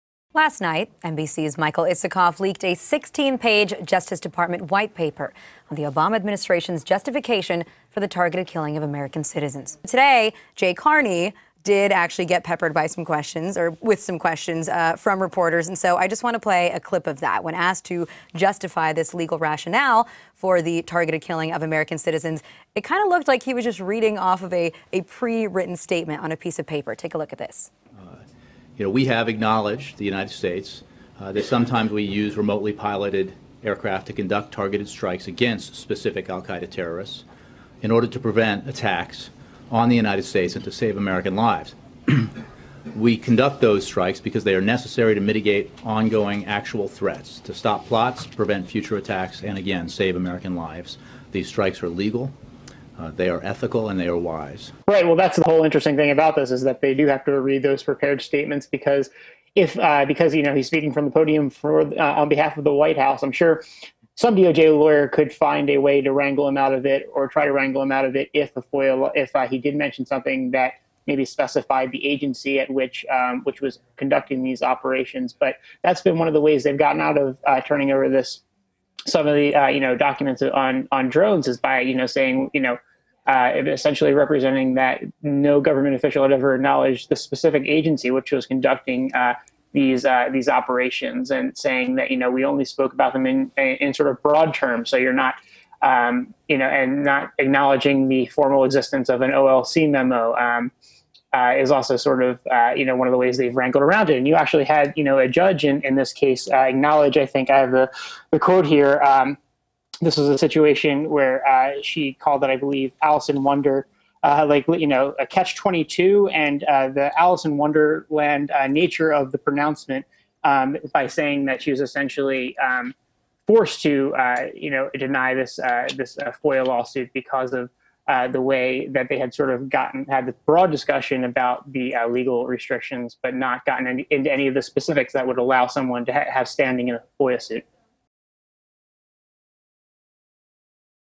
Jay Carney: Drone Strikes Are 'Legal,' 'Ethical' And 'Wise'
NBC has leaked a Justice Department 'white paper' on the Obama administration's justification for the targeted killing of American citizens, and Jay Carney has defended their use.